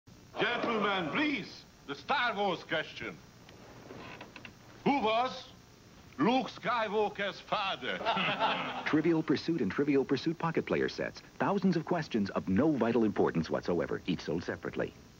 Trivial Pursuit & Trivial Pursuit Pocket Player Board Games Ad (1987) (low quality)
Commercial for the Trivial Pursuit and Trivial Pursuit Pocket Player board games from 1987. Low quality.